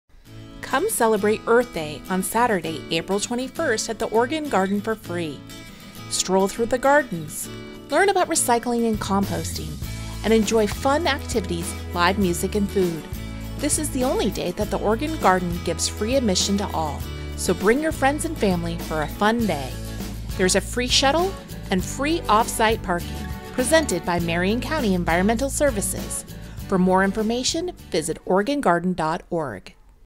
2018 Earthday Celebration at the Oregon Garden
Come join us when we celebrate Earth Day (49 years and going strong!) at the Oregon Garden. Interactive booths, music, food, fun and a chance to visit the most beautiful garden in Oregon. all for free!